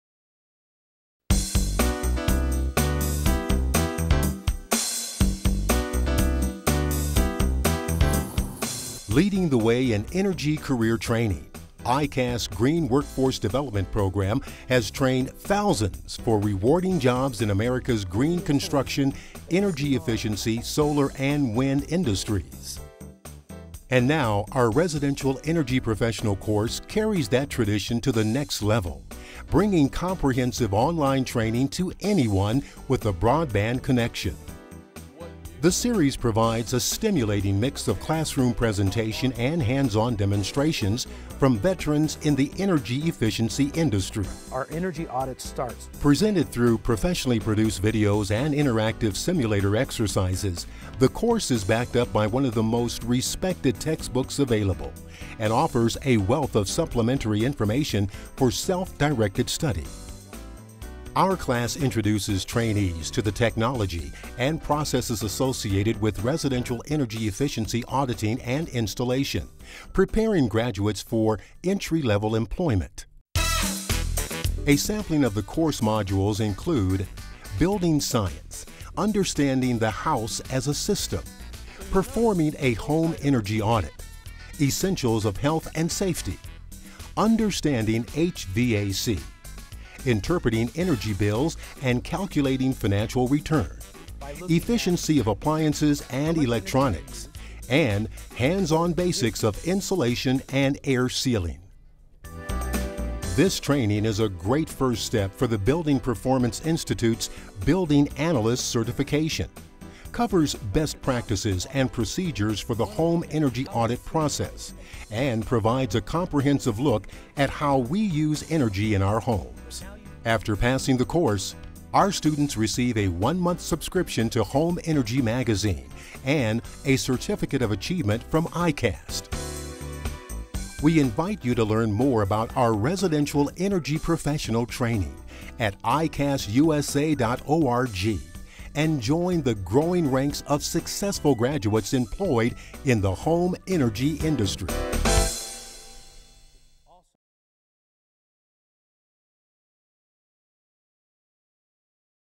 ICAST - Residential Energy Professional Course Intro
This online course prepares students with backgrounds in construction, electrical, plumbing, HVAC, as well as IT, marketing, sales, administration, and operations on how to perform a residential energy audit. Imparted knowledge includes building science and the home as an integrated system.